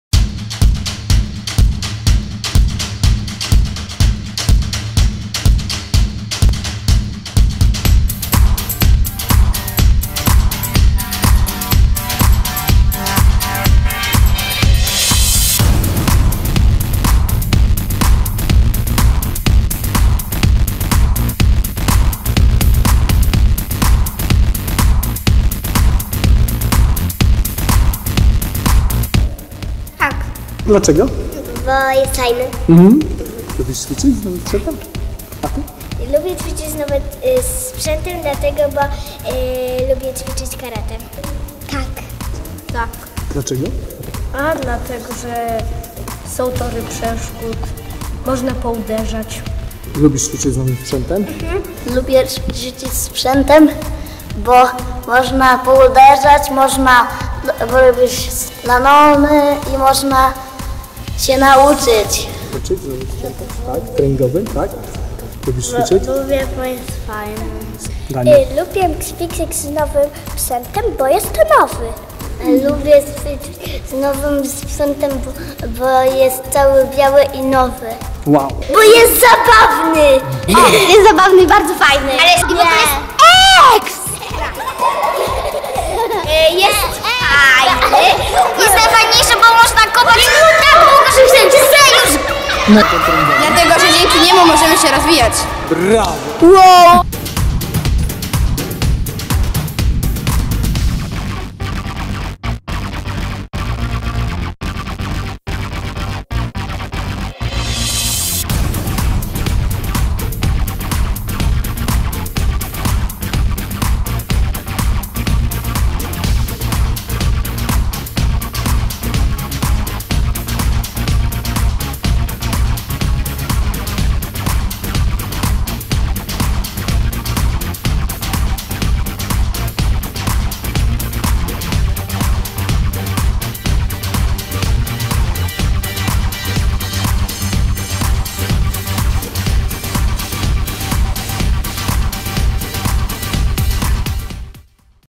Klub Sztuk Walk SOKUDO Otwock- Mały i duży karateka w akcji - treningi z FIO Mazowsze Lokalnie
Film przedstawia treningi realizowane w ramach projektu "Mały i duży karateka w akcji" oraz jak zakupiony nowy sprzęt z dotacji "Funduszu Inicjatyw Obywatelskich – Mazowsze Lokalnie”  pozytywnie wpłynął na treningi w Klubie Sztuk Walk "Sokudo" z Otwocka.